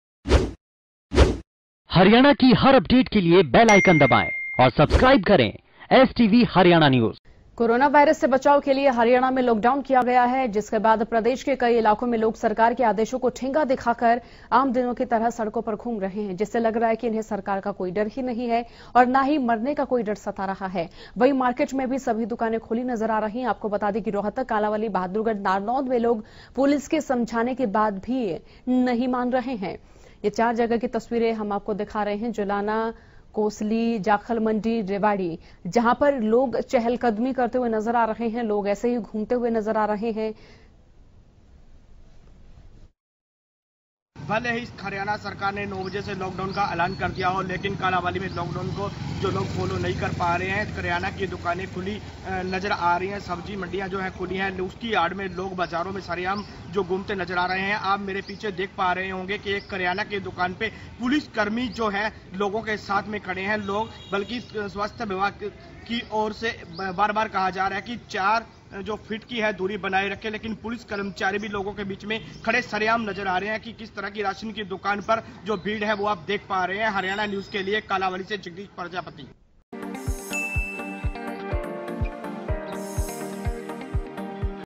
Corona Virus:प्रदेश मे लॉकडाउन! कई इलाको में लोग कर रहे नियमों का उल्लंघन || STV Haryana News
कोरोना वायरस से बचाव के लिए हरियाणा में लॉक डाउन किया गया है... जिसके बाद प्रदेश के कई इलाकों में लोग सरकार के आदेशों को ठेंगा दिखाकर आम दिनों की तरह सड़कों पर घूम रहे है जिससे लग रहा है कि इन्हें सरकार का कोई डर है और ना ही मरने का... वहीं मार्किट में भी सभी दुकानें खुली नजर आ रही है... बता दे कि... रोहतक, कालांवली ,बहादुरगढ़, नारनौंद  में लोग पुलिस के समझाने के बाद भी नहीं मान रहे है...